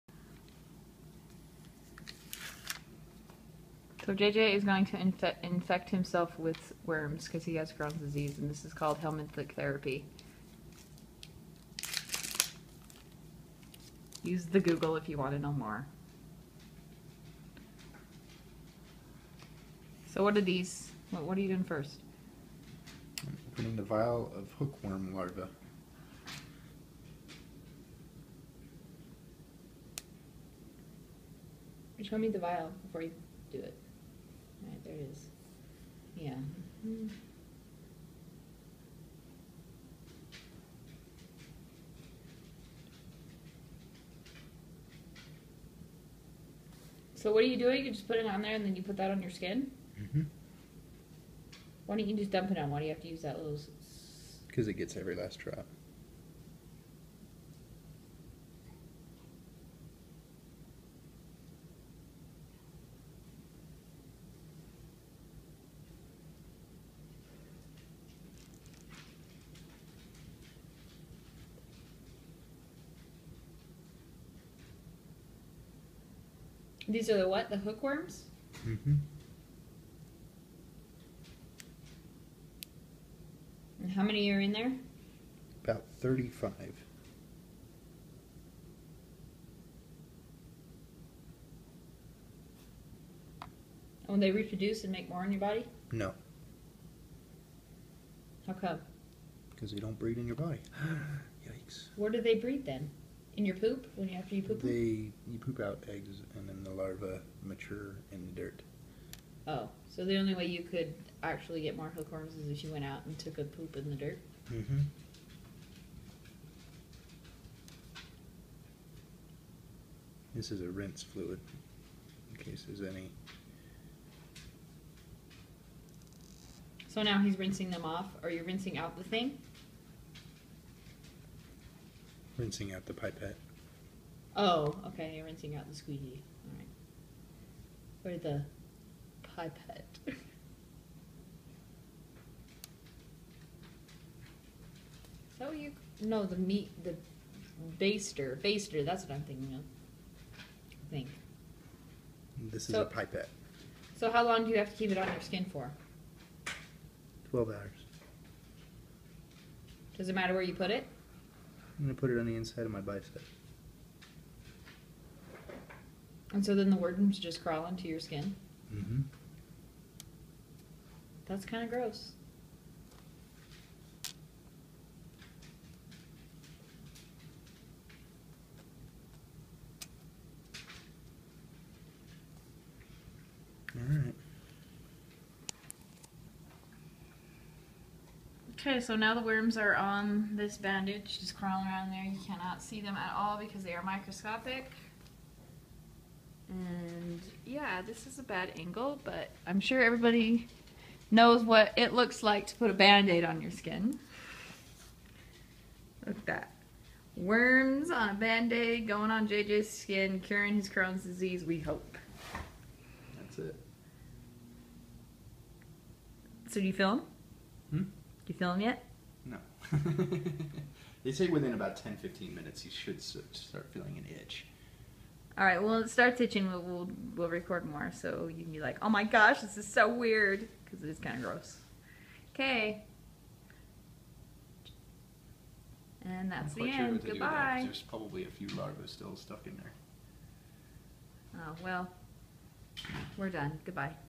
Hookworm Helminthic Therapy Self Infection
My husband has the autoimmune disease: Crohns Disease. Here he is infecting himself with hookworms.

*I'll come back later and explain my husbands condition. If you're reading this, I haven't got around to it yet. Basically, this is our last resort. I hope it works!